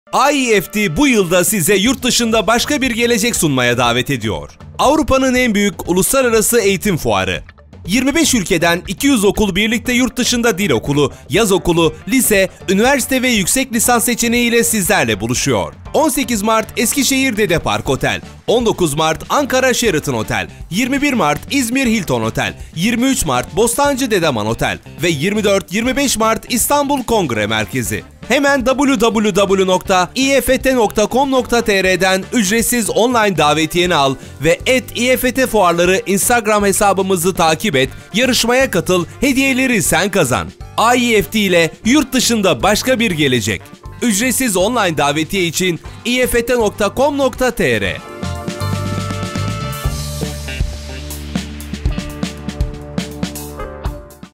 IEFT ile Yurtdışında BAŞKA BİR GELECEK! 18-25 Mart 2018
Avrupa'nın En Büyük Uluslararası Eğitim Fuarı IEFT 18-25 Mart'ta İstanbul, Ankara, İzmir, Eskişehir'de.
Şimdi ücretsiz online davetiye al, yurtdışında başka bir geleceğin kapılarını arala

Instagram hesabımız : @ieftfuarlari/

24-25 Mart İstanbul Kongre Merkezi
23 Mart İstanbul Bostancı Dedeman Otel
21 Mart İzmir Hilton Otel
19 Mart Ankara Sheraton Otel
18 Mart Eskişehir Dedepark Otel